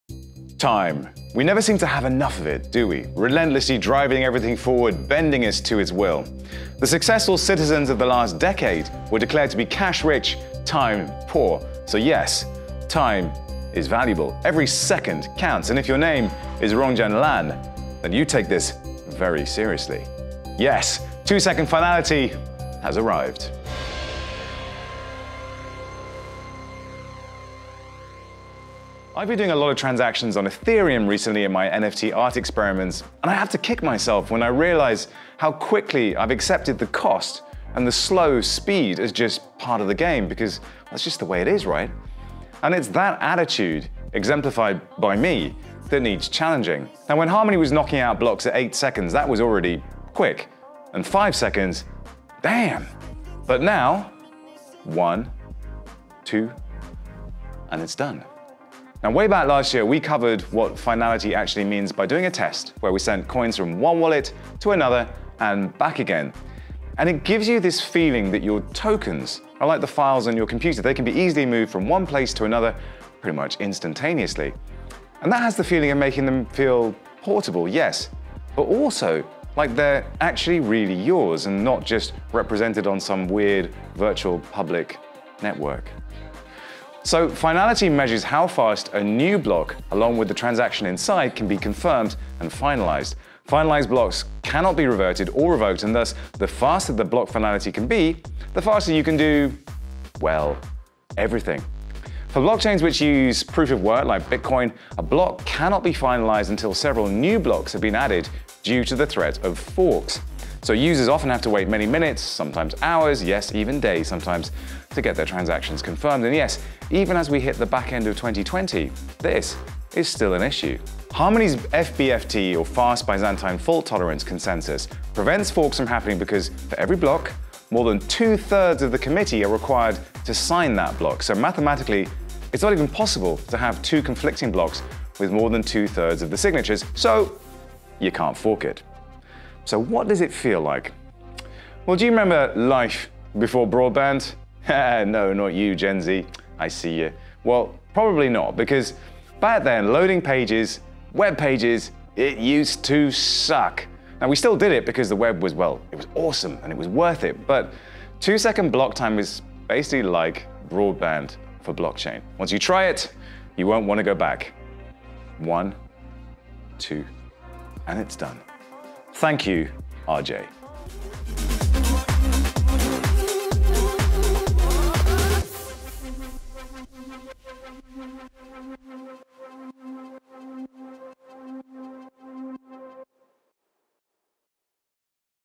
2 Second finality is here
If you head to the block explorer right now. You will find that the block latency now presents as 2 seconds. It's faster than ever and as cheap as it ever was.